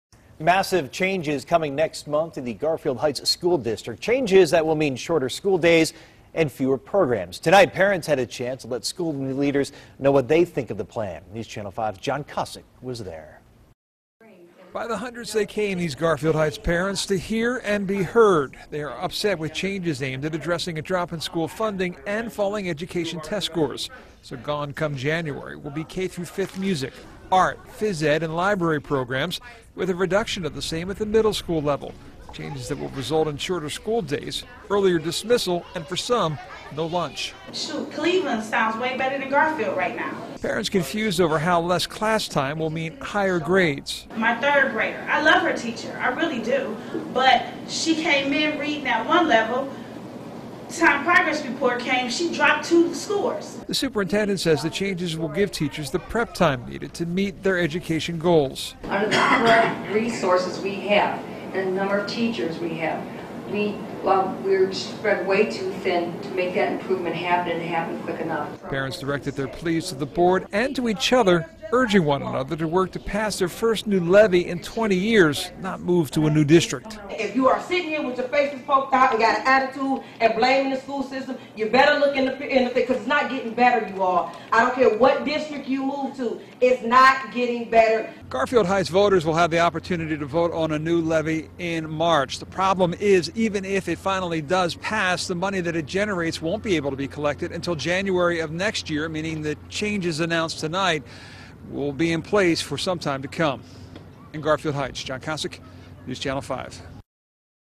Garfield Hts. Cuts
John Kosich reports on the changes coming to Garfield Hts. schools.